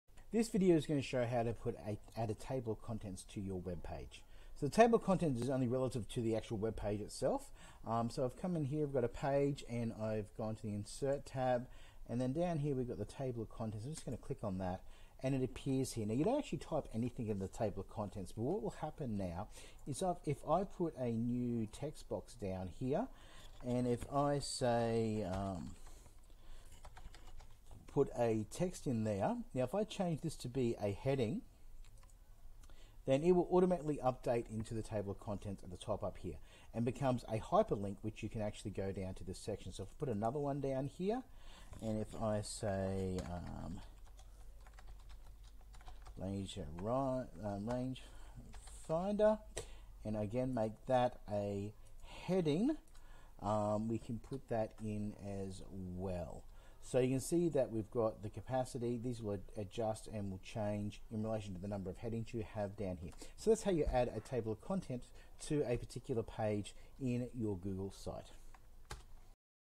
Google Site - Adding a table of content
How to add a table of content to a page in your Google Site